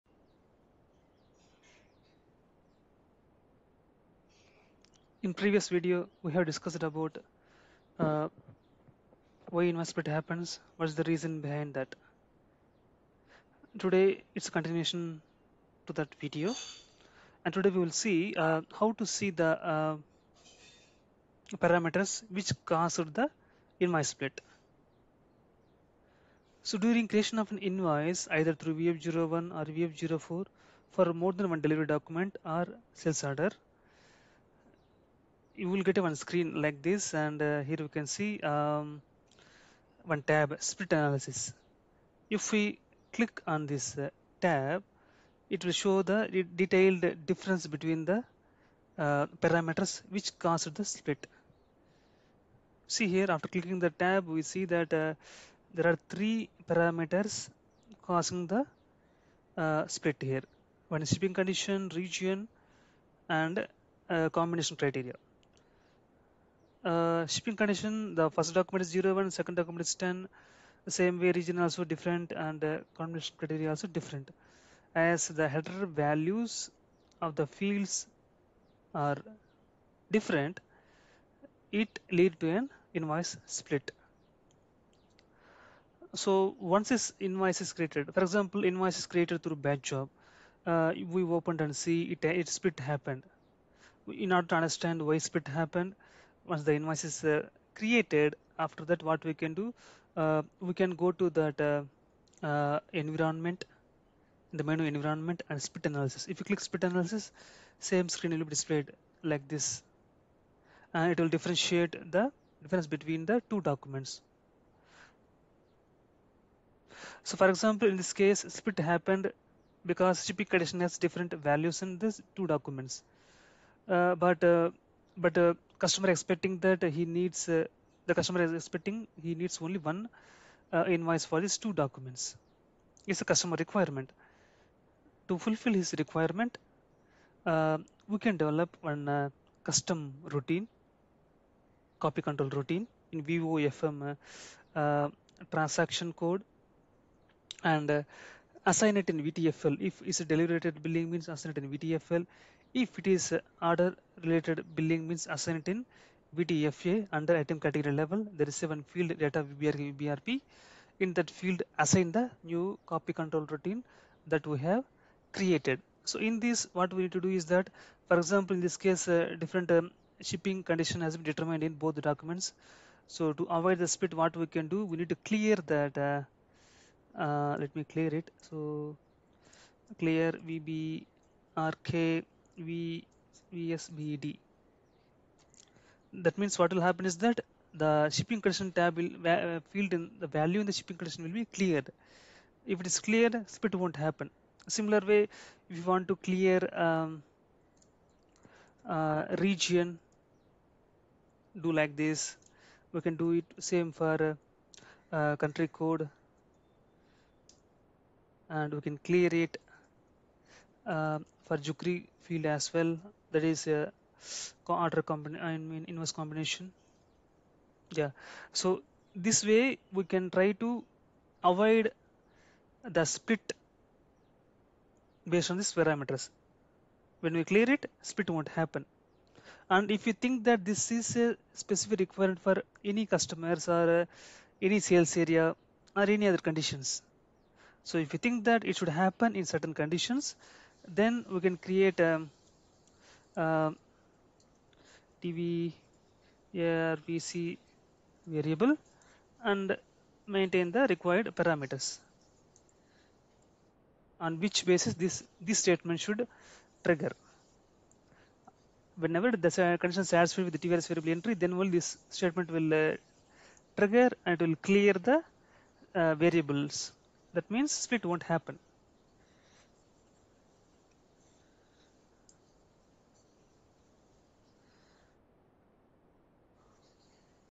Invoice split criteria in SAP SD (Split Analysis)
In this session, SAP SD AND ABAP LEAGUE SUPPORT Raj is going to discuss about Invoice split criteria in SAP SD - (Split Analysis). How to do split analysis in VF02/VF03 and what are the steps that we need to follow etc. And, how to avoid split by clearing some fields are discussed in this session Invoice split criteria in SAP SD (Split Analysis). And this is part 2 on this invoice split.

Watch out complete video till end to get more clarity on invoice split criteria.